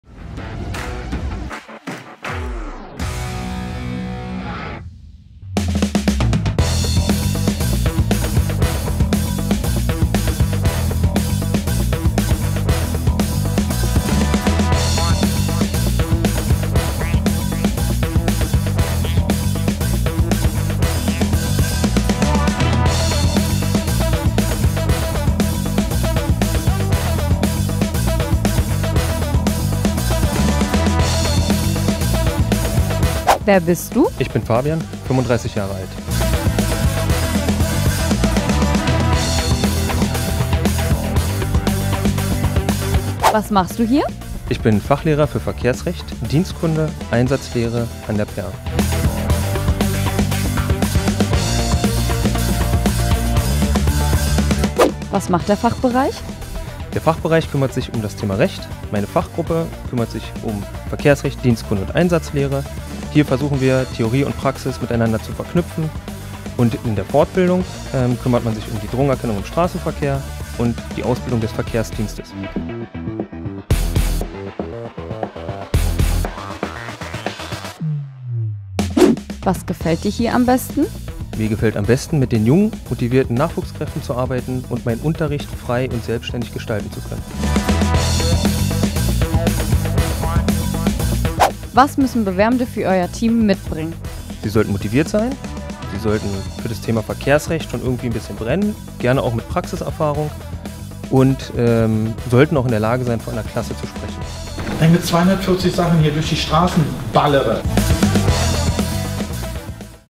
Unsere Polizeiakademie - Teil 9 - Verkehrsrecht
Wir zeigen euch in regelmäßigen Abständen und in 110 Sekunden unsere Fachausbilderinnen und Fachausbilder, die unsere Azubis mit viel Liebe und Engagement zu guten Polizistinnen und Polizisten ausbilden. On top bekommt ihr auch einen kleinen Einblick in die Ausbildungsinhalte der einzelnen Fachbereiche. Im 9. Teil zeigen wir euch die Fachgruppe I 2 – Verkehrsrecht.

Viel Spaß und bleibt dran!